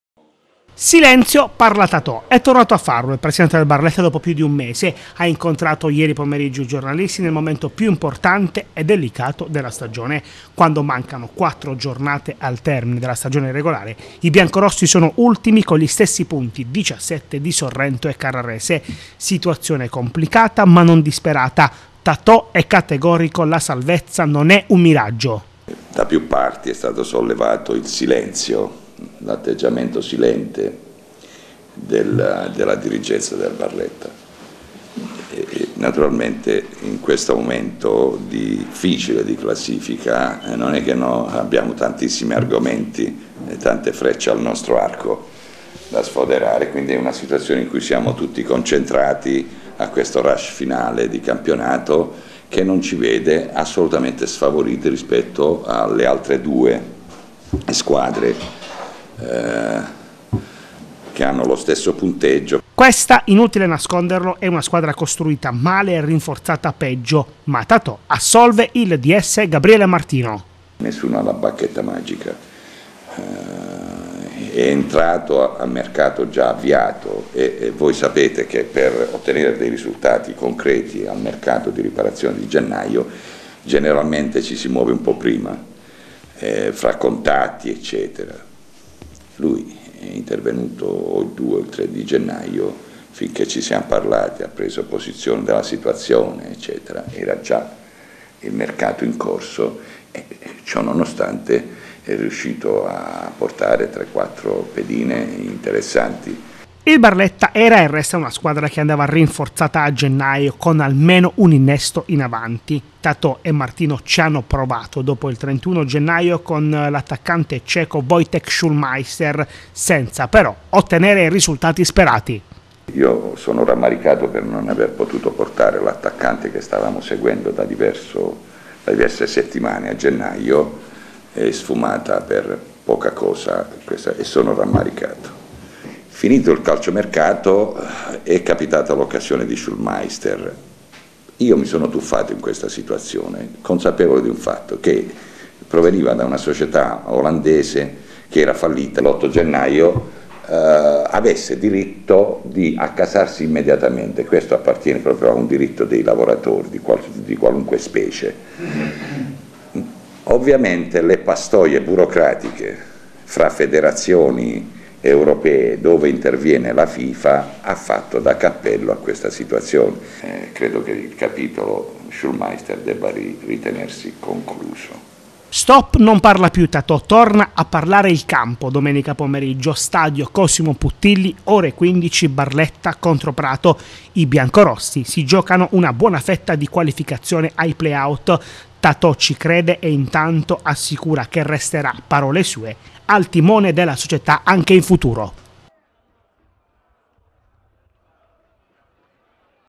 Barletta Calcio: Roberto Tatò rompe il silenzio. Salvezza possibile
Il presidente del Barletta Calcio, Roberto Tatò, è tornato a parlare ieri, in conferenza stampa, dopo un lungo silenzio. Ha detto di credere ancora nella salvezza e assolve il direttore sportivo Gabriele Martino. Domenica, intanto, c'è la delicata sfida contro il Prato.